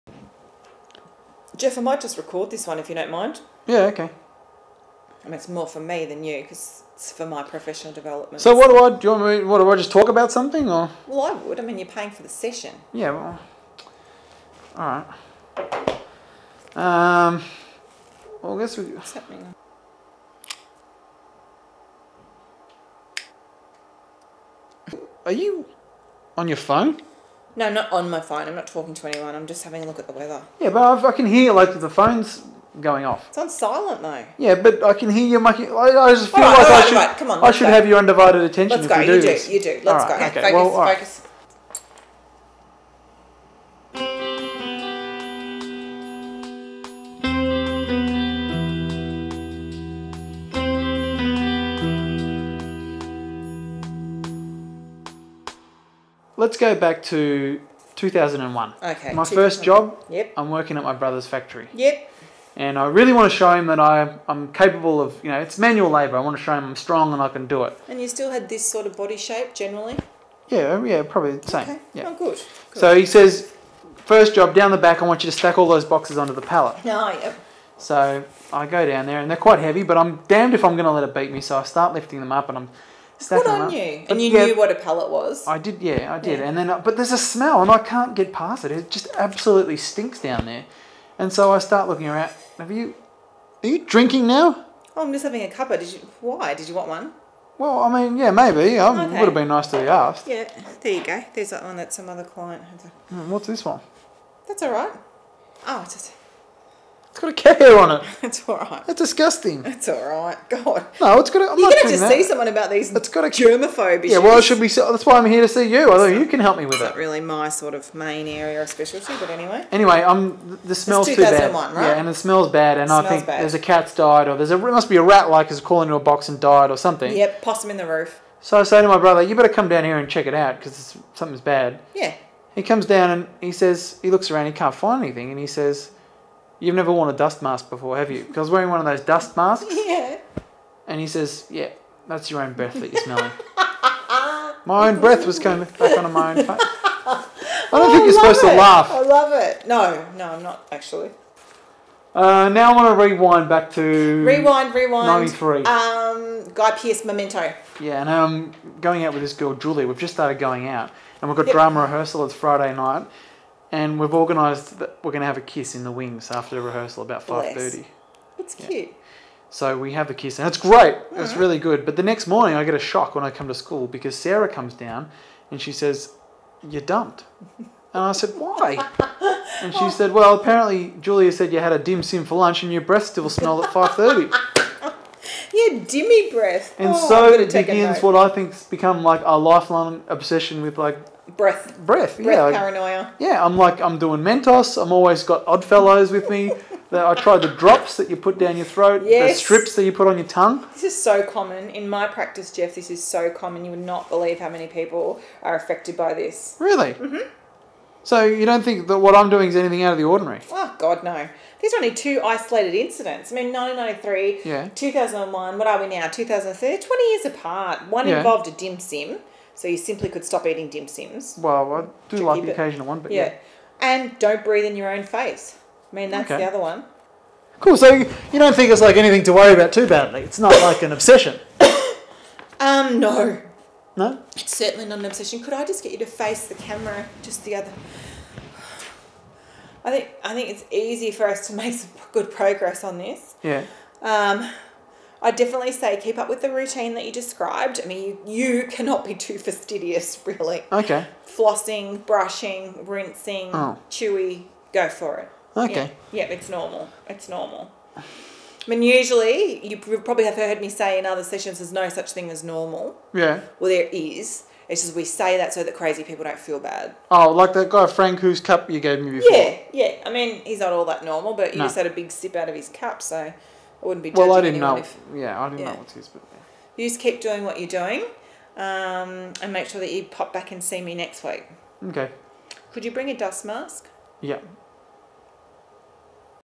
HASH it OUT - Jeff's breath paranoia
Jeff discusses how he has never gotten over being dumped in year 9 for having 'dimmy breath'